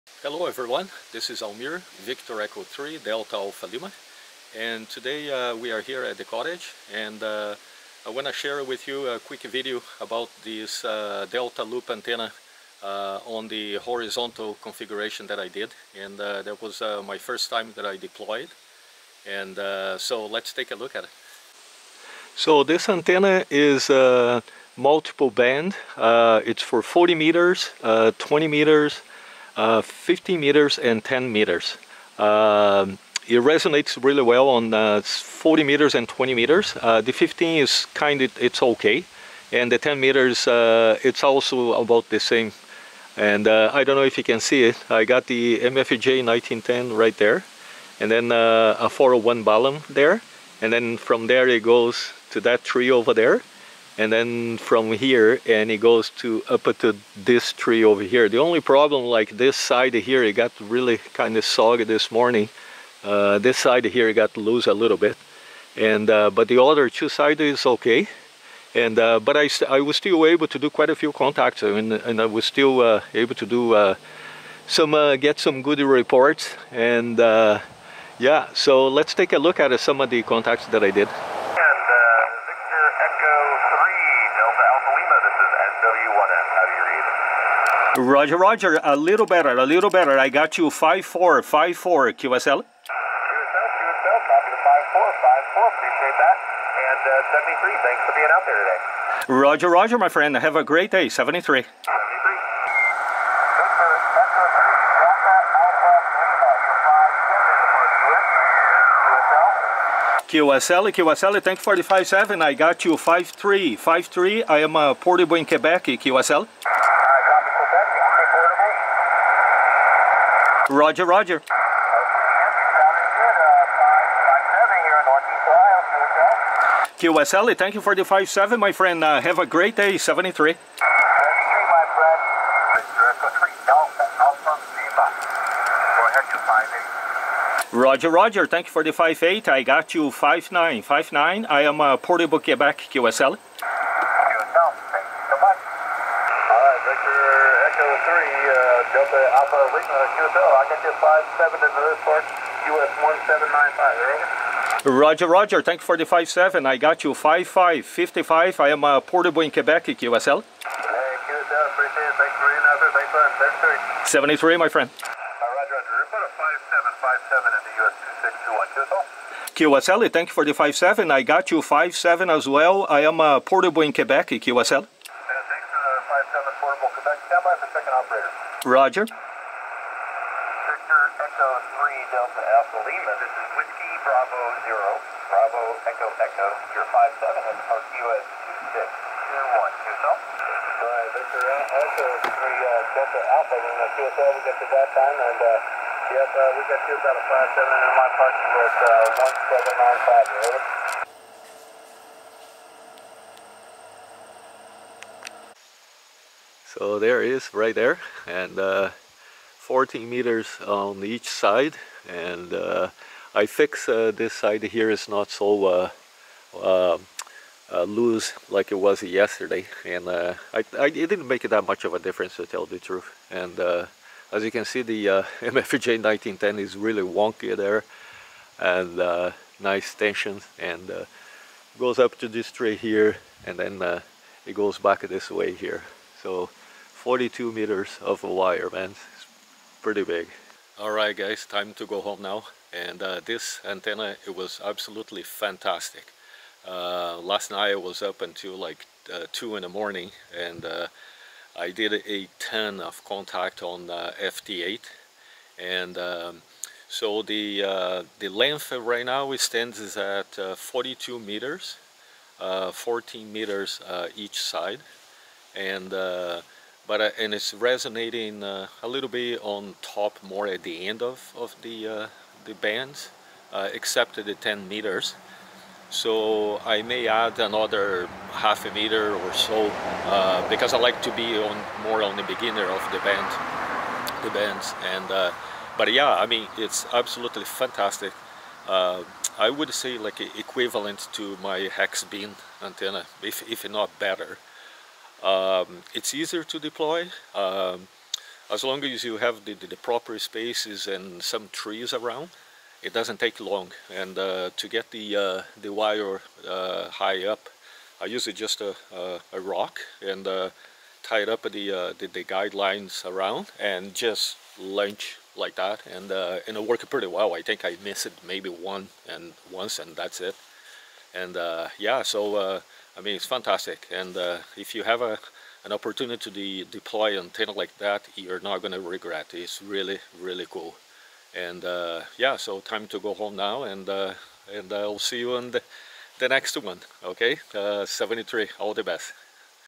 HAM RADIO - Multiband Delta Loop Antenna Horizontal Plane 40m, 20m, 15m, 10m
I was really happy by the results from this antenna. It will be on the top of my favourite list for sure. The length of the wire is 42 meters divide by 3 which is 14 meters. I will increase the size by a half meter or so.